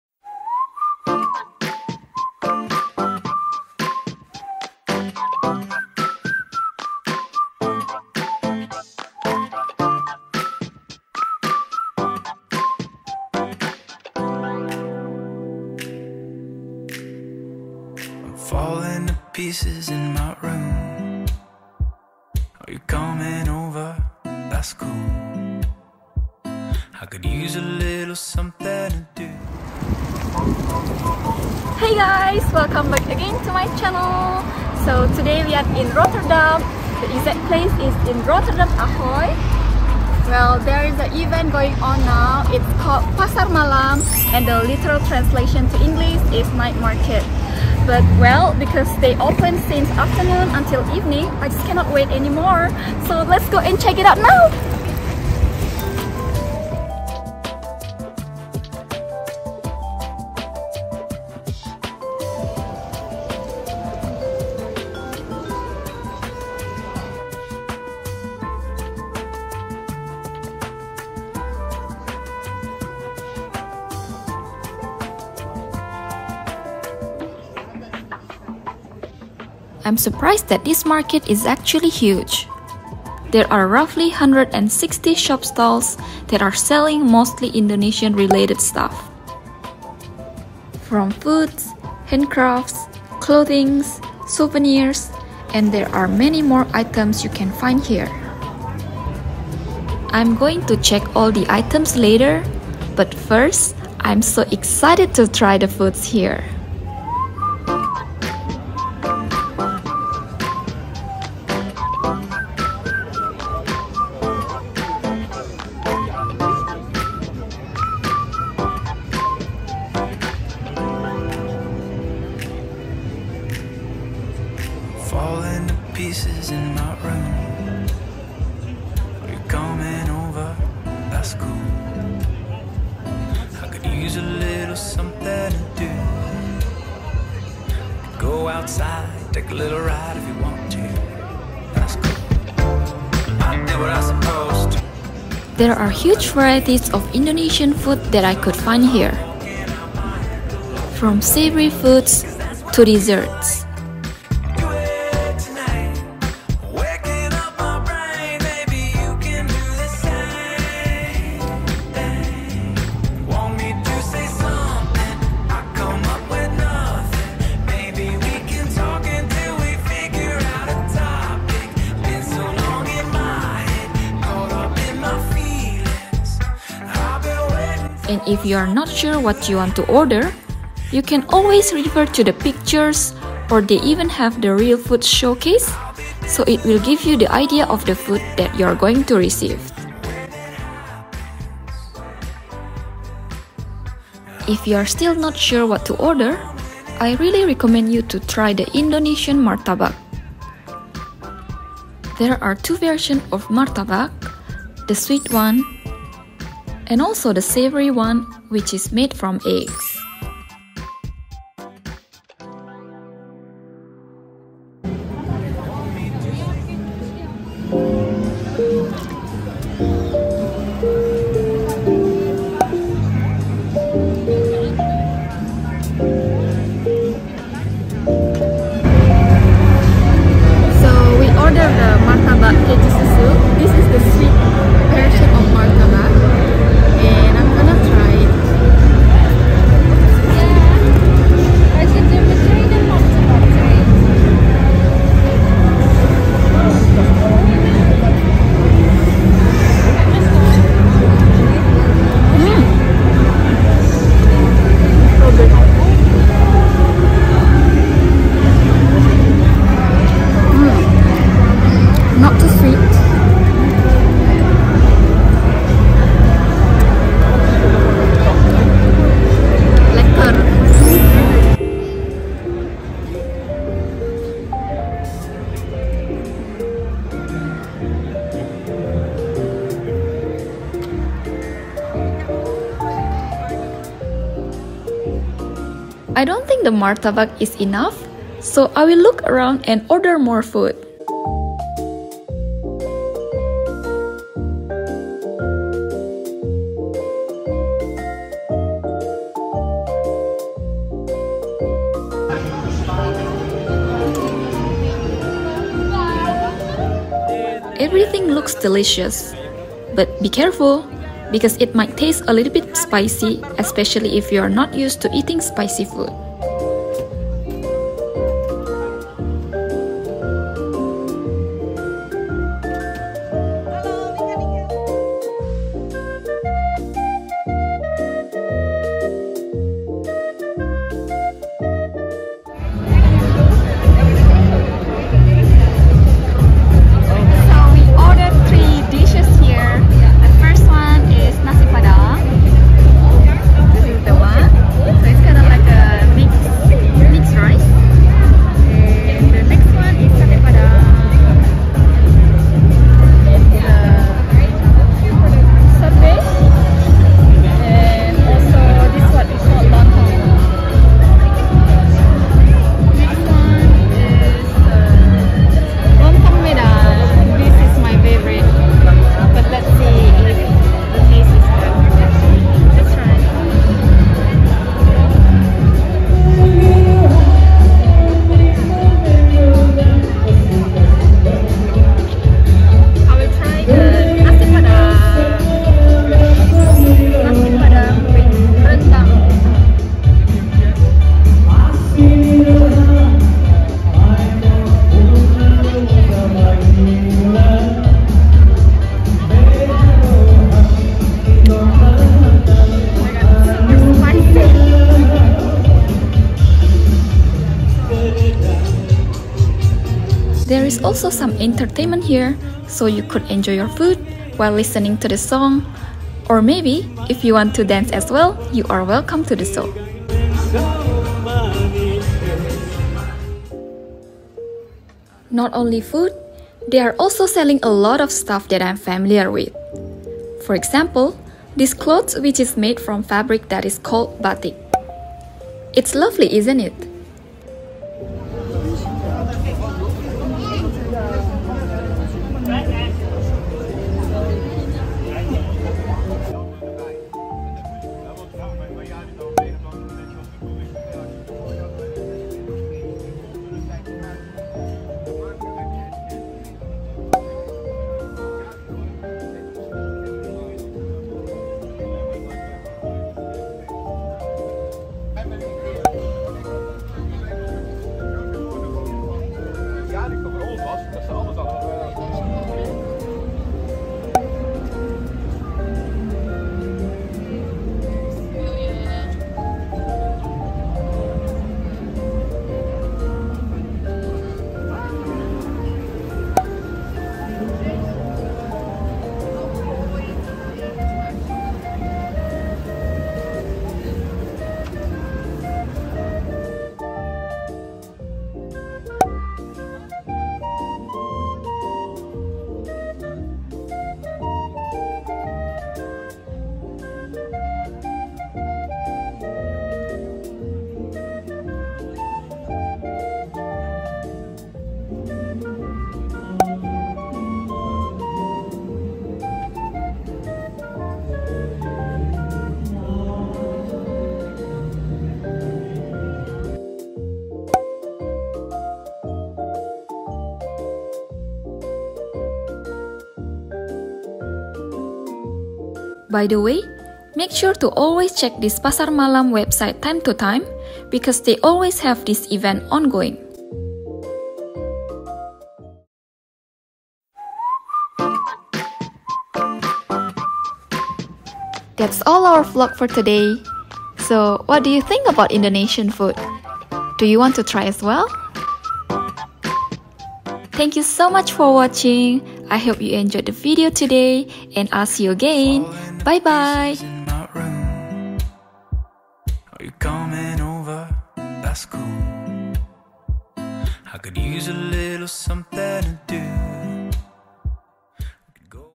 Indonesian Food Market in Europe!🇳🇱 (Rotterdam, Netherlands) | Pasar Malam Istimewa
English🇺🇸  (Scroll ke bawah untuk versi Bahasa Indonesia🇮🇩 )

Hello!
I went to an Indonesian night market which is called "Pasar Malam Istimewa" this month, it was held in Rotterdam (The Netherlands)

I was very excited to try the food there, especially there were a lot of varieties of Indonesian food! everything looked so good, It was tough to choose which one to order😅

also, if you are planning to try Indonesian food, I have some recommendations for you to try, so make sure to check my video 😊

Not only food, they were also selling a lot of Indonesian related stuff like clothes, souvenirs, snacks, etc.

Bahasa Indonesia🇮🇩

Halo!
Bulan ini Aku berkesempatan untuk pergi ke Pasar malam Indonesia yang namanya "Pasar Malam Istimewa".
Lokasinya waktu itu ada di Rotterdam (Belanda)

Aku semangat sekali untuk cobain makanan disana, terutama karena makanan Indonesianya banyak sekali pilihannya. Semuanya kelihatan enak, sampai bingung mau milih yang mana😅

Mereka nggak hanya menjual makanan, tapi juga banyak menjual barang-barang yang berhubungan dengan Indonesia, seperti baju, souvenir, snacks dan lainnya.

Semoga kamu suka dengan videonya!

My other videos that might interest you 🎬
(Video yang mungkin menarik untuk kamu)

NETHERLANDS VLOG (VLOG DI BELANDA)

My very FIRST FIFA World Cup Experience!⚽️⚽️⚽️
(Pengalaman nonton piala dunia pertamaku!)

Music 🎵

Tags 🍬